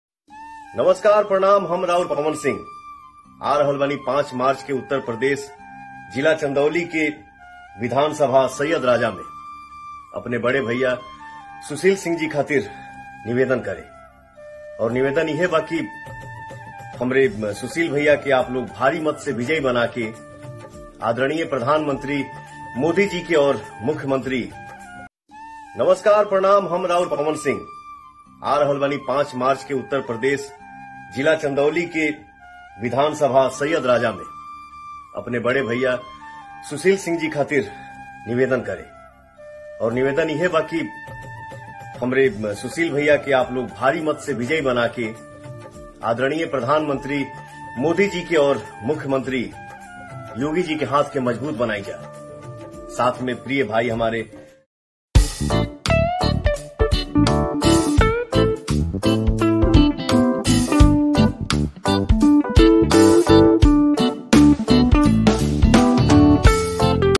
पवन सिंह ने कि बहुत बड़ी अपील 5 तारीख को क्या धमाका करेगे पवन सिंह
पवन सिंह ने कि बहुत बड़ी अपील 5 तारीख को क्या धमाका करेगे